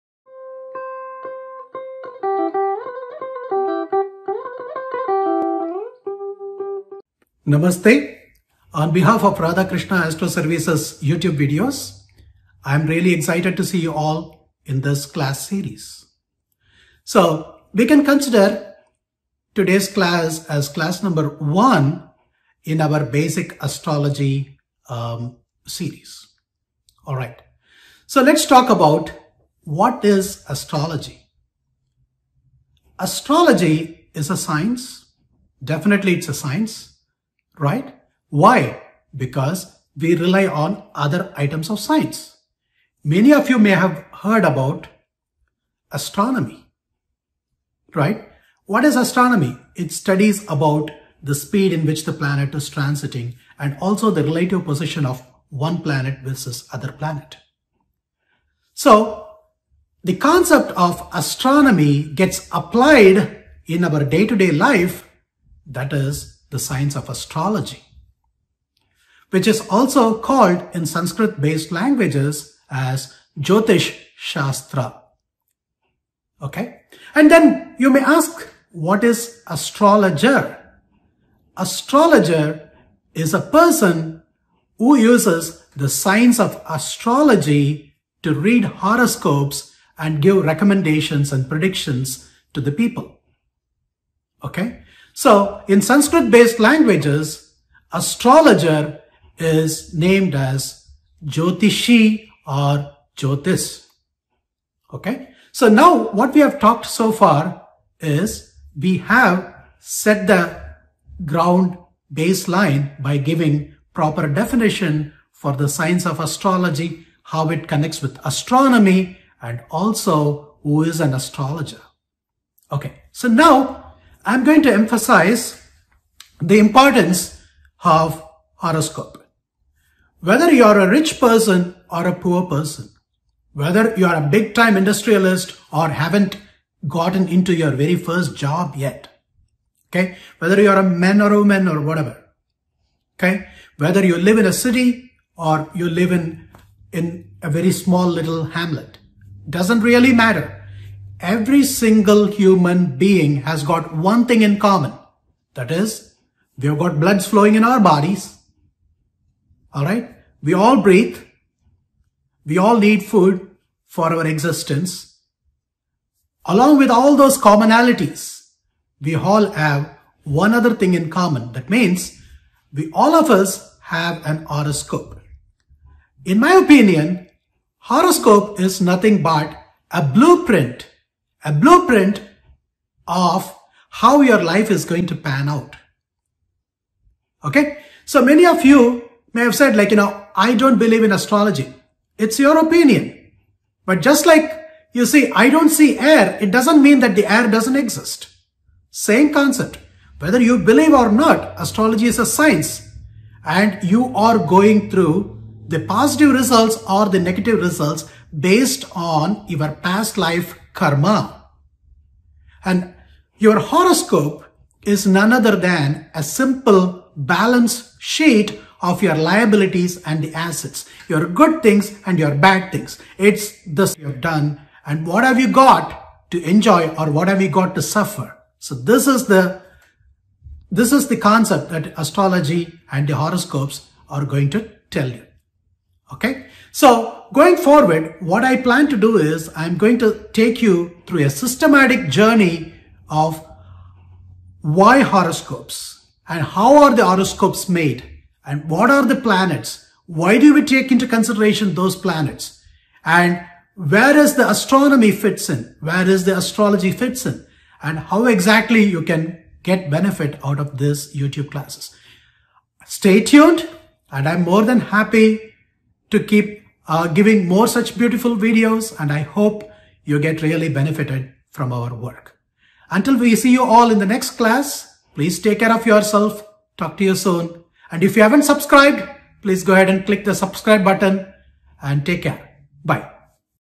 Astrology: Who is an Astrologer? What are their duties?
Astrology is a divine science handed down through the Vedas. According to the Sanathana Dharma principles the Vedas are a vast body of knowledge that covers every aspect of human life from Science, Technology, Sociology, Politics, Economics, Medicine, etc.,

This video is our first class on the Fundamentals Principles of Astrology.
The Sanskrit name for Astrology is "Jyotisha Shastra". Astrologer is referred as "Jyotishi" or "Jyotish" in Sanskrit. Here's a Vedic mantra: tamasoma jyotirgamaya. Jyotihi means light. Just like the light removes the darkness the Astrology is the science to help human beings live happily and upon exhausting their Karma achieve liberation.

You may approach an astrologer to receive guidance, overcome confusion, or to seek clarification on life events. Rich or poor; villager or city dweller; man or woman; healthy or sick; no matter whoever a person is they all have a horoscope. Whether one accepts the Astrological principles or not. The science of Astrology exists.

Natal horoscope is a snapshot of the planets  in the space and their relative position with the earth at the time of one's birth. Using horoscope the Karmic trajectory of a person could be studied. Astrologer gives proper guidance based on one's horoscope. We urge everyone to systematically understand the science of astrology.

Dr. K. Balasubramaniam from Radha Krishna Astro Services (RK Astros) explains how astrologer guides people to cross the ocean of material darkness. He has got over 28 years of personal experience in astrology. He has served people from all walks of life across all the continents of the world. Through this initiative Dr. Balasubramaniam is eager to teach the wonderful art and science of Astrology for the benefit of one and all. This video is the first in the series of the educational videos delivered in Tamil language.